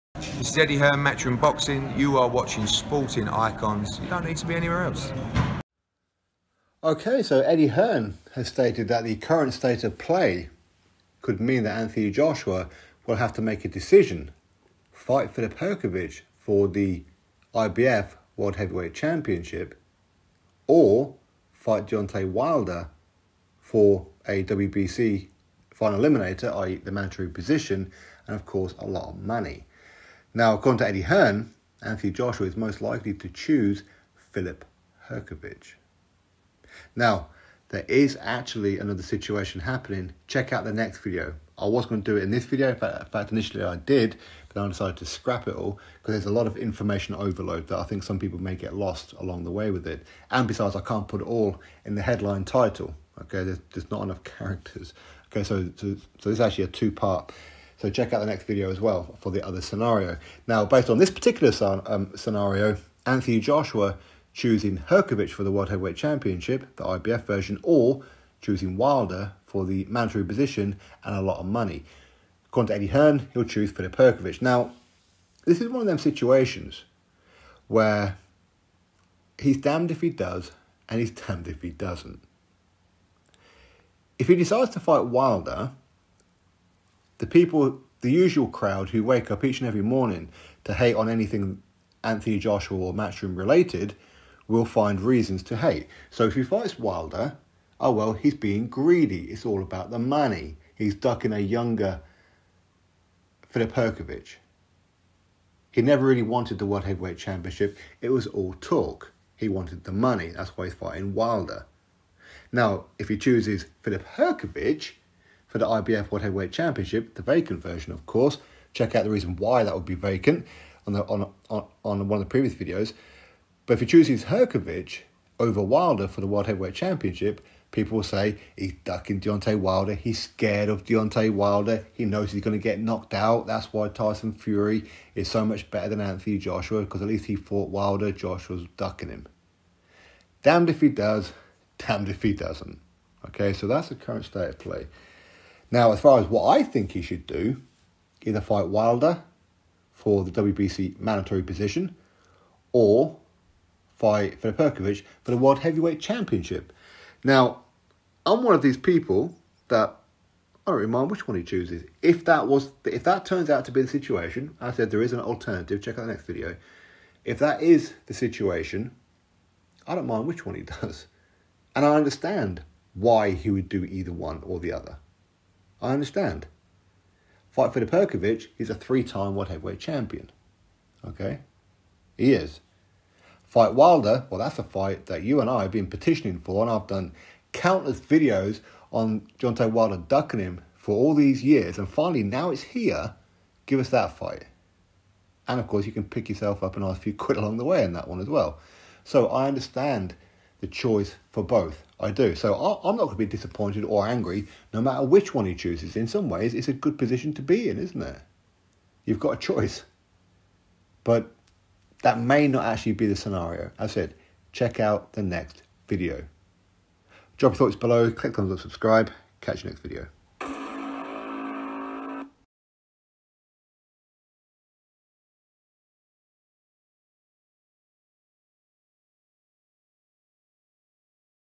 Anthony Joshua to CHOOSE Filip Hrgovic OVER Deontay Wilder says Eddie Hearn!!
Latest Boxing News: Anthony Joshua to CHOOSE Filip Hrgovic OVER Deontay Wilder says Eddie Hearn.